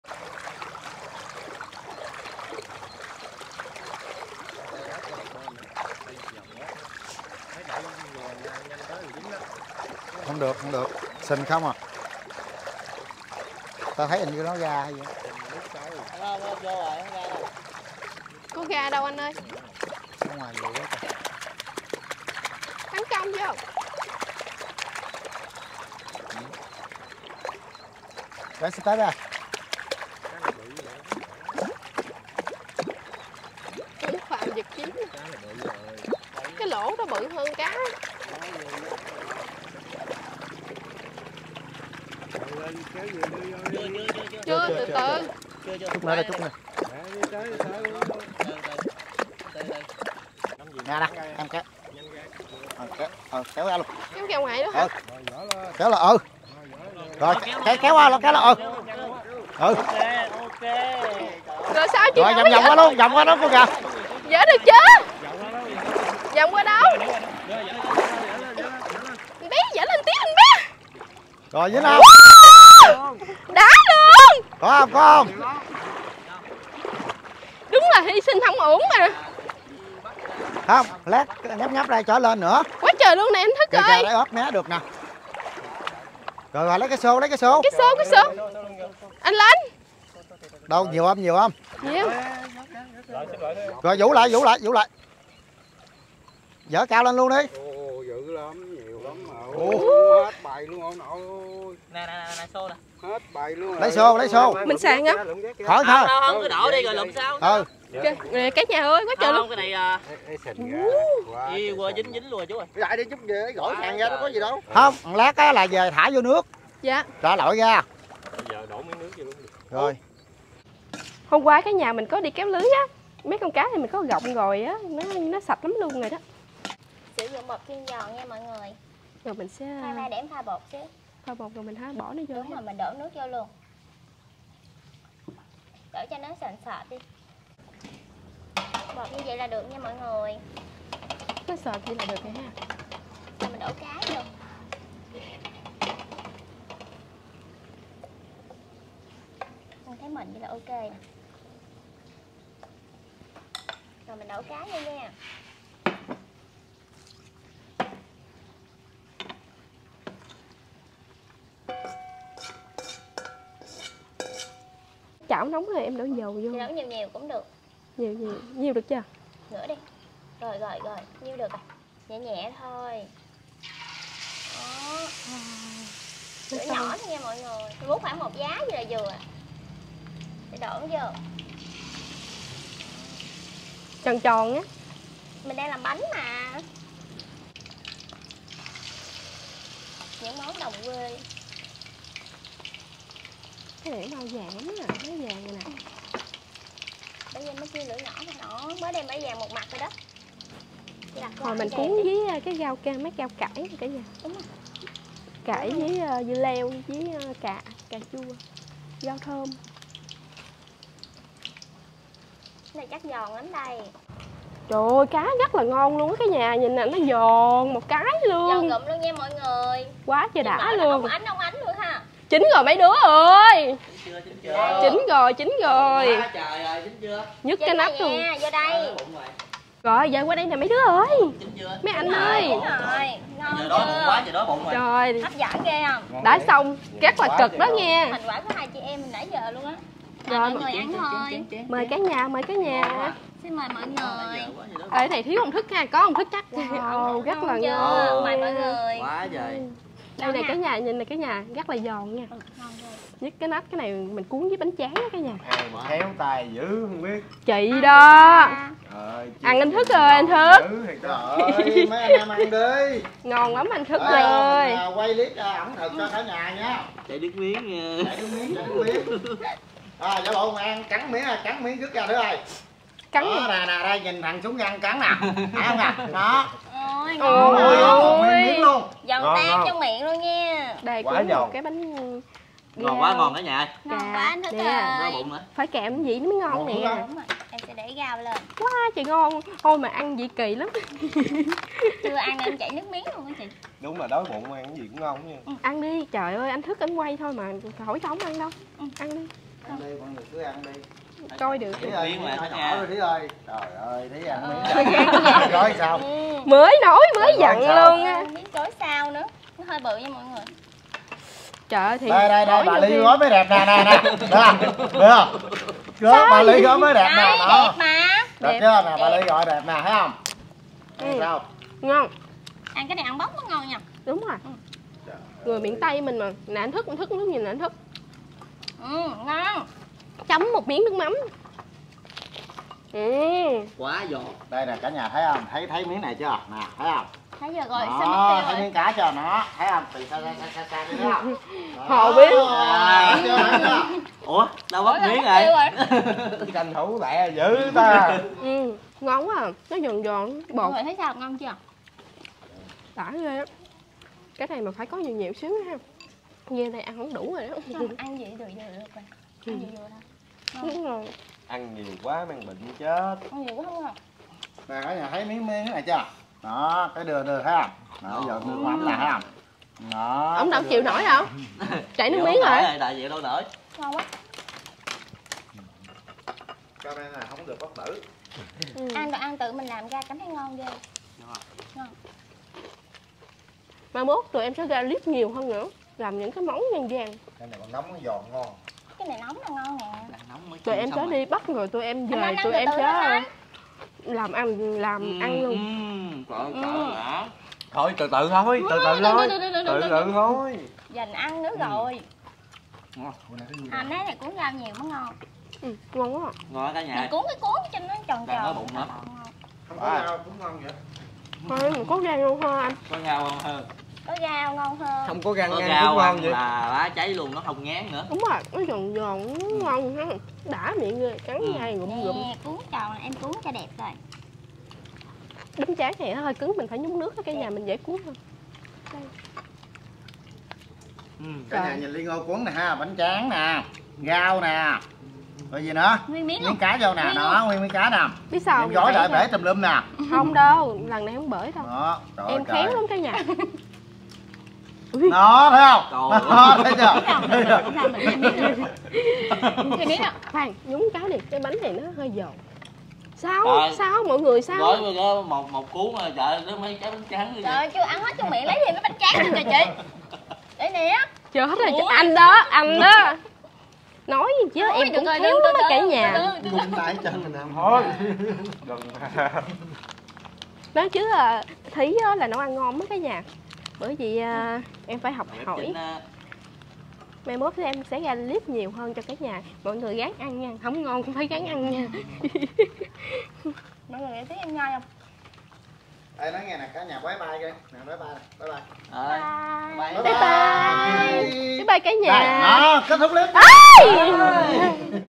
Kéo Lưới Bắt Cá Cùng Ly Ngô, Cá Chiên Bột Ăn Cực Giòn | Ly Ngô Vlog
Mình cảm ơn.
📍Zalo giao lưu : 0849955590
Mình cảm ơn mọi người ! Chúc cả nhà luôn vui vẻ hạnh phúc 🤎